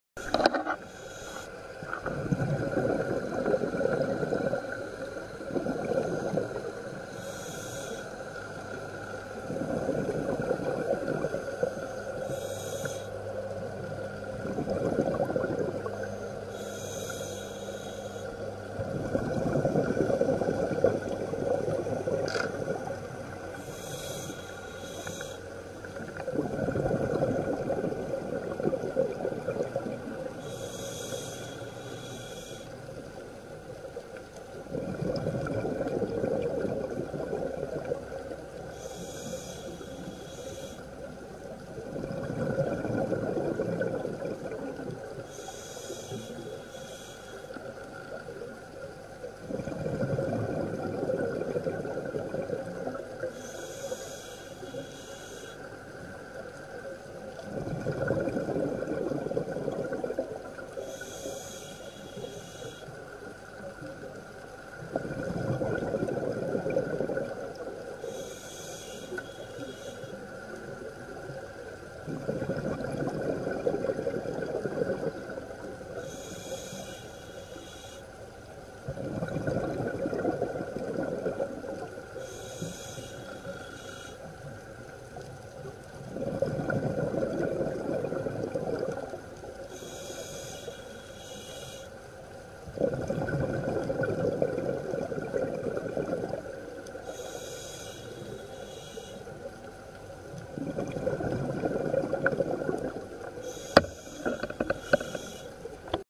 Crab with a Leaf House
This leaf was moving, so I took a closer look. The little eyes sticking out were a giveaway, so I looked underneath. Wow! I certainly didn't expect to find this Crab. It wasn't happy and I felt bad for disturbing it, but I wanted to see what it would do. After a few failed attempts to turn the leaf back over, it decided to go around and get underneath again. Off it went! Fantastic!